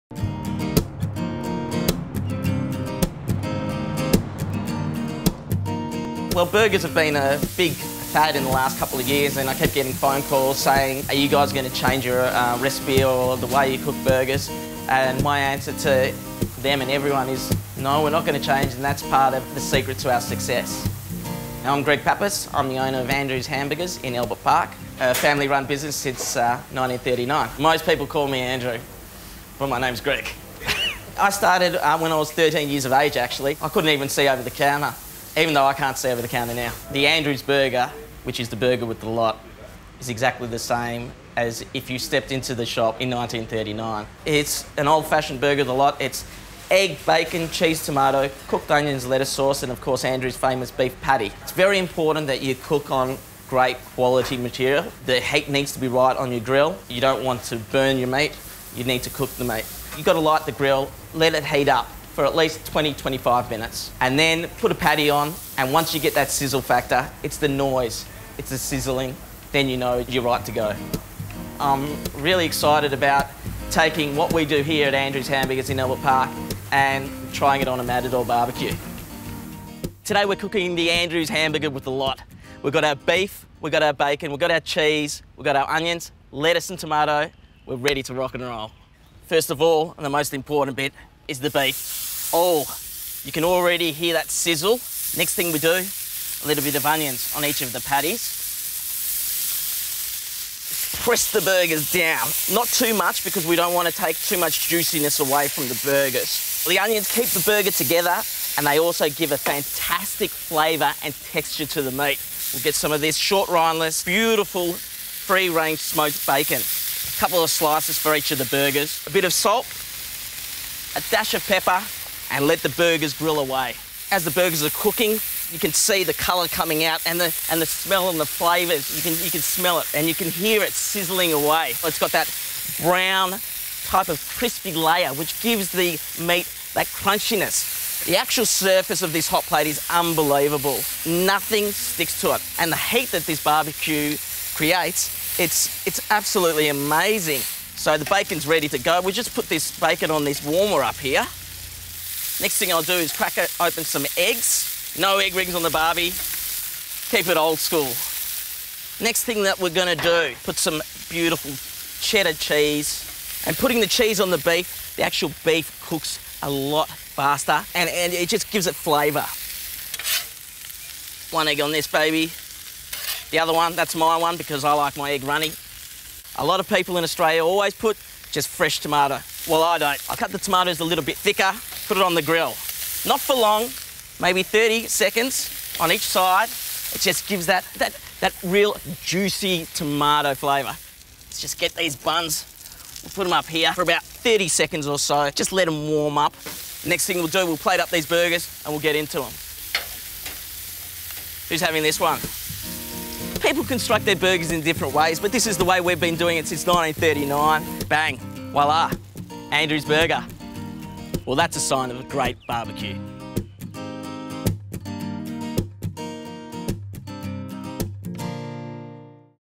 Matador BBQ's meets Andrews Hamburgers - Matador BBQ's
Matador BBQs have gone out on the road to learn the many secrets of cooking a great barbie. Thanks to Andrews Hamburgers in Albert Park for teaching us how to make the perfect burger with the lot.

Discover more tips, tasty recipes and all the signs of a great Matador BBQ